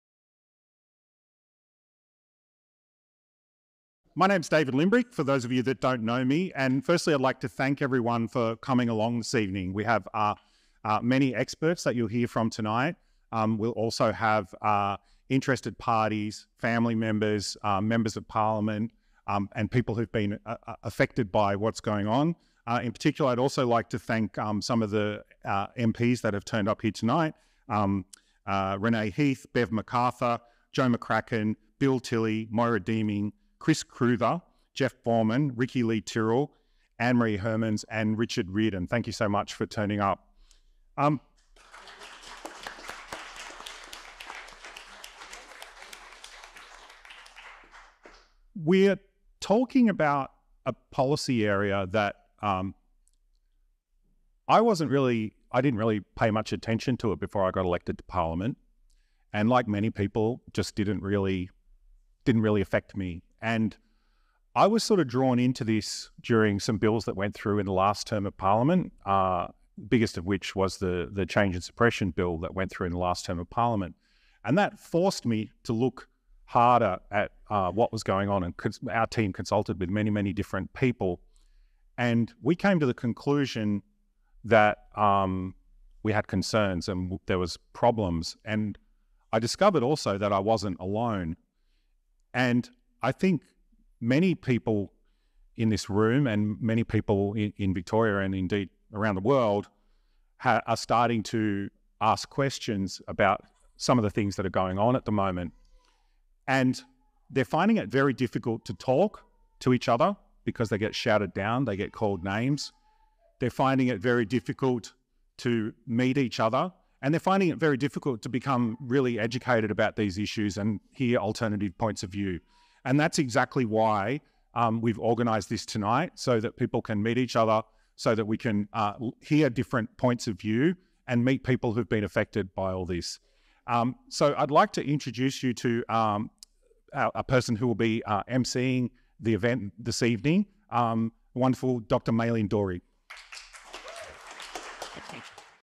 Child Safeguarding & Consent Forum - David Limbrick MP
Child Safeguarding & Consent Forum: Delving into the short and long term implications of medical and pharmaceutical interventions for children with gender dysphoria.

David Limbrick MP, Victorian Legislative Council

Parliament of Victoria, 3 October 2023.